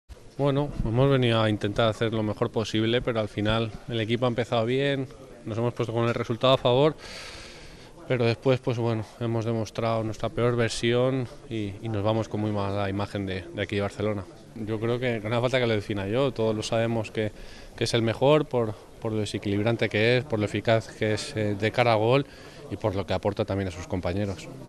Soldado: "Hemos demostrado nuestra peor versión"
y El jugador del Valencia ha lamentado el partido que han jugado contra el Barcelona y ha dicho que "hemos demostrado nuestra peor versión".